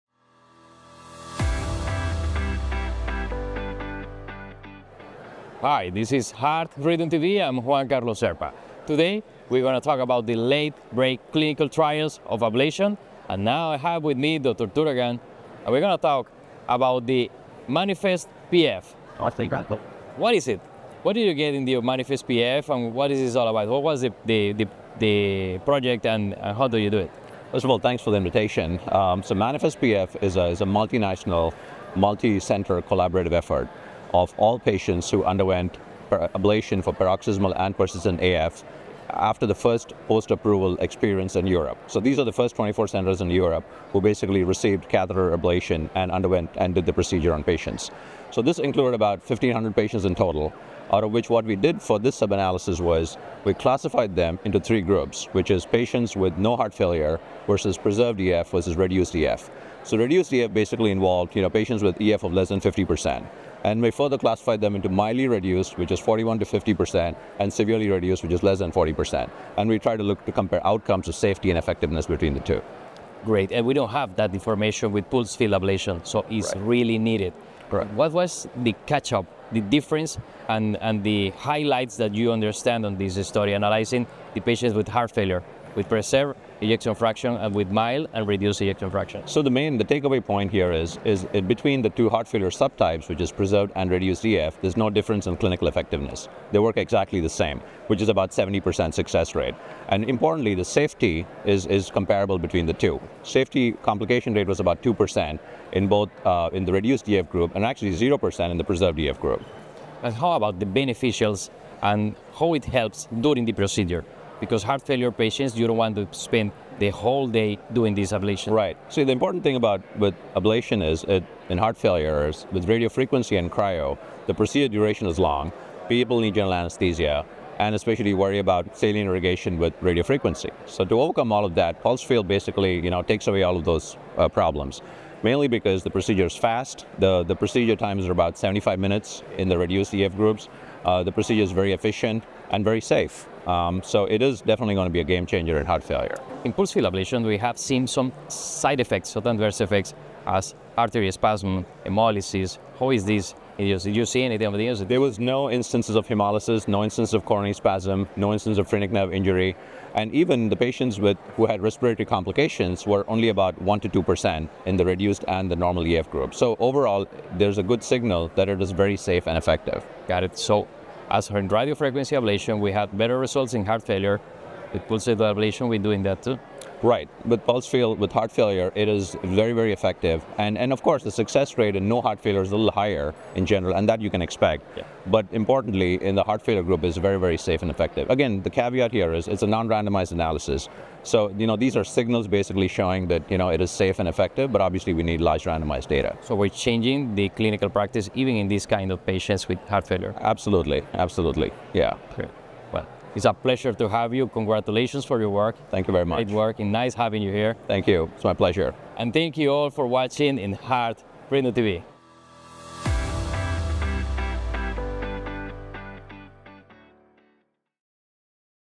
Heart Rhythm TV Update: A MANIFEST-PF Registry Sub-analysis
Immediately following the LBCT presentation at HRS2024, Heart Rhythm TV host Juan Carlos Zerpa Acosta, MD, sat down with Mohit K. Turagam, MD, FHRS, to discuss Safety and Effectiveness of Pulsed Field Ablation for Atrial Fibrillation in Patients with Heart Failure: A MANIFEST-PF Registry Sub-analysis.